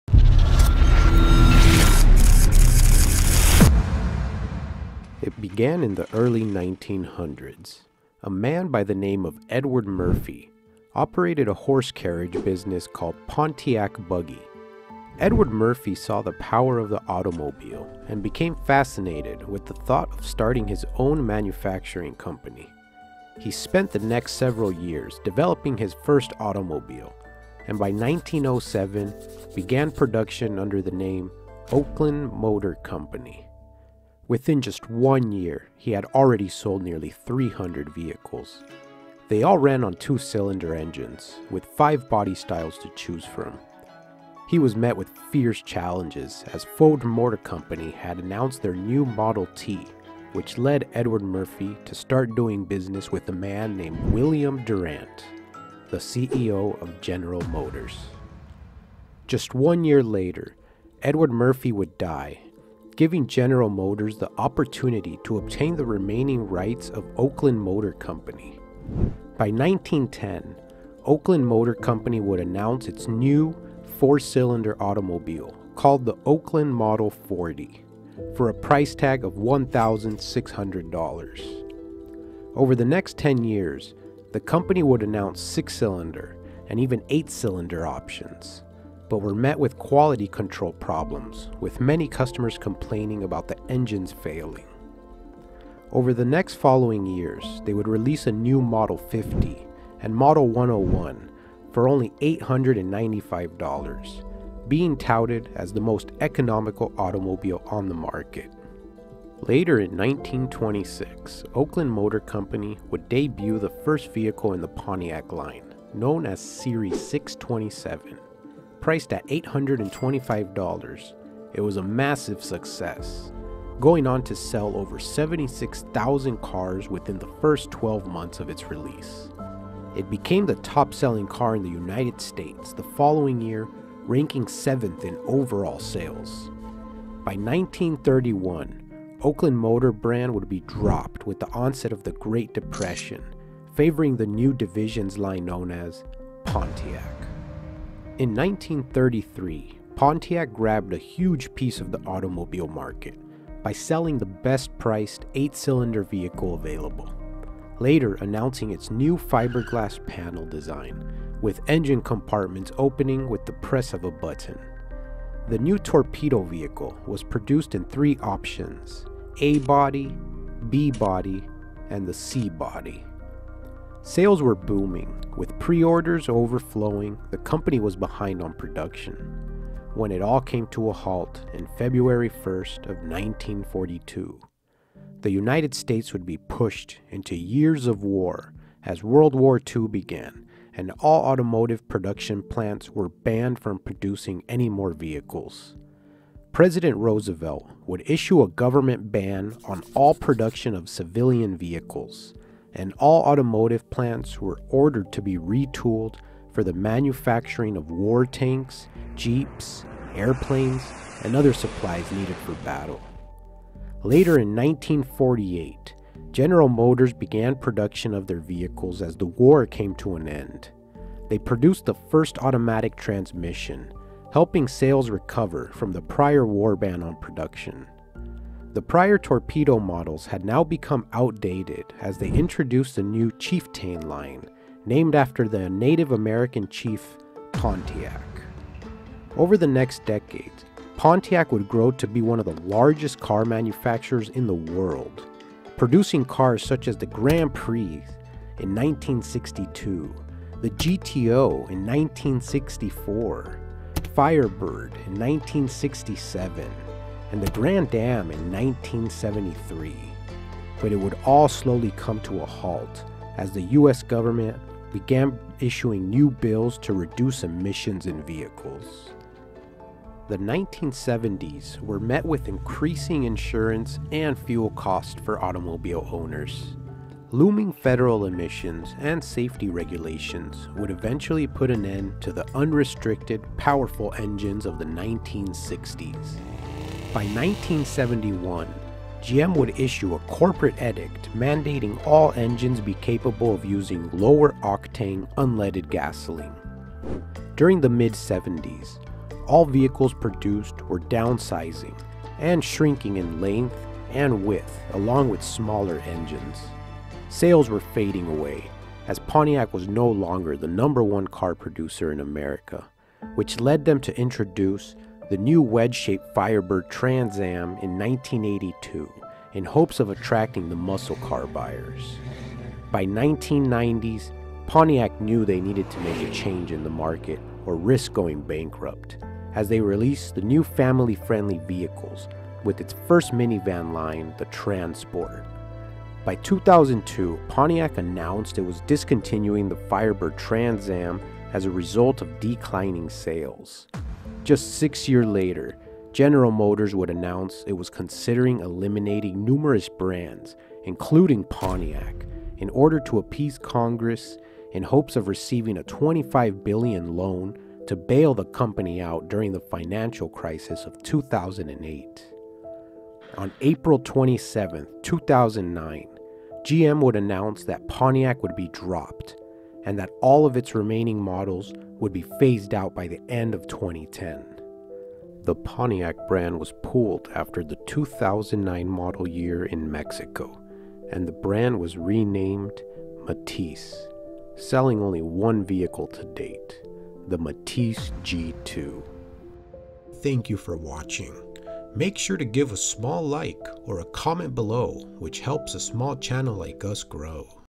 What Happened to Pontiac?
From becoming one of the top automobile manufacturers in America, Pontiac is now left as a piece of American History dating back to the early 1900s. We discuss the Rise and Fall of Pontiac Motor Company.